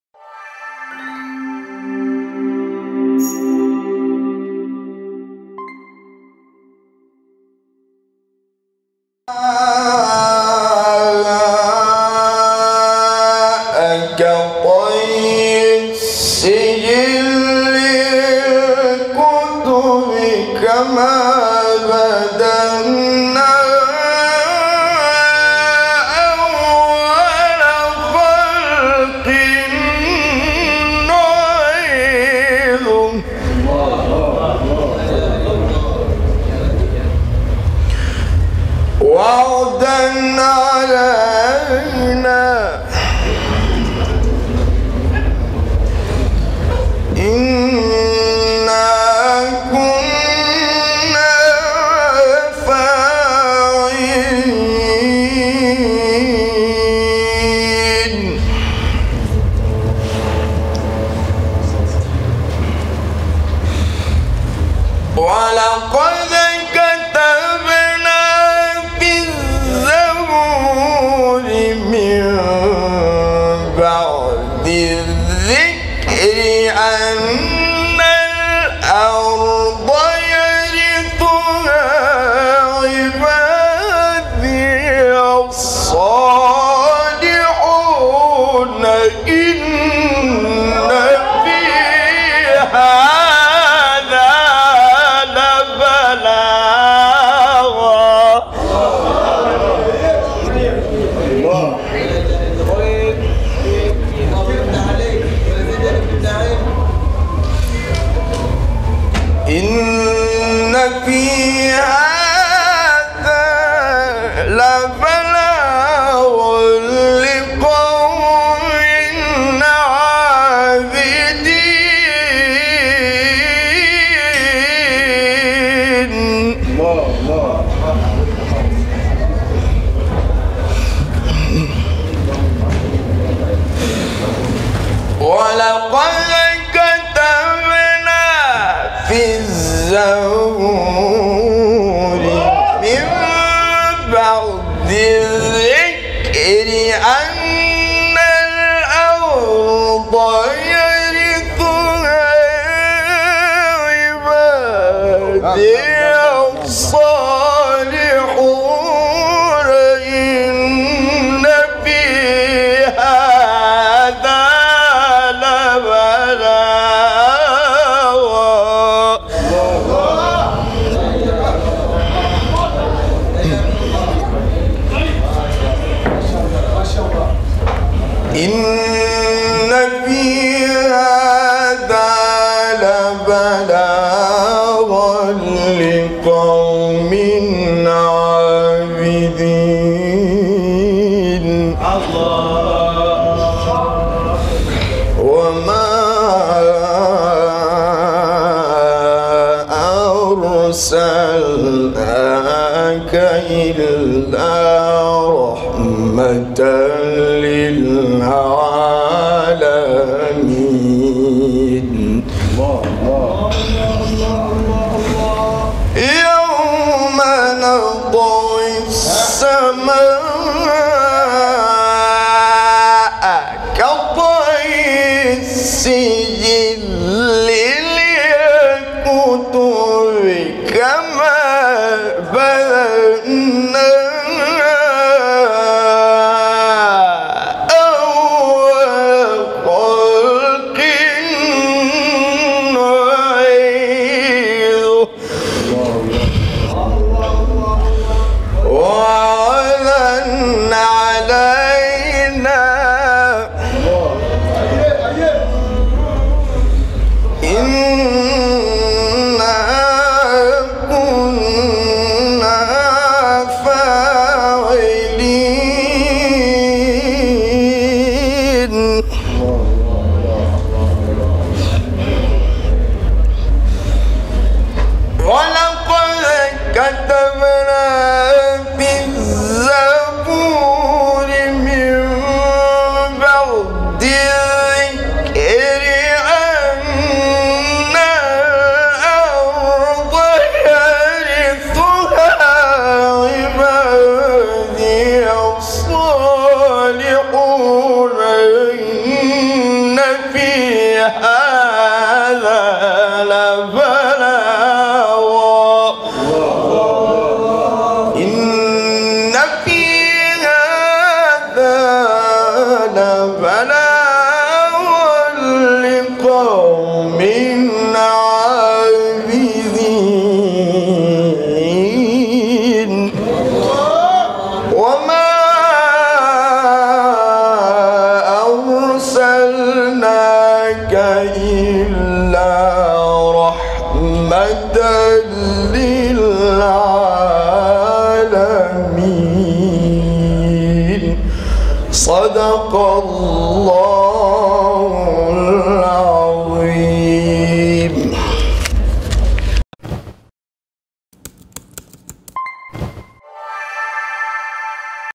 Best Recitation - Qur AN Al Karym / Qari Ustadh Juma Kinyasi
Mashaa Allah kazi kubwa sana mwamba Juma Kinyasi akisoma kwa uzuri na ubora wa hali ya juu kabisa  hafla ya kuwafariji watoto yatima katika ukumbi wa lamada ilala jijin Dar es Salaam tizama video zetu mpaka mwisho usisahau kushare

And
Thanks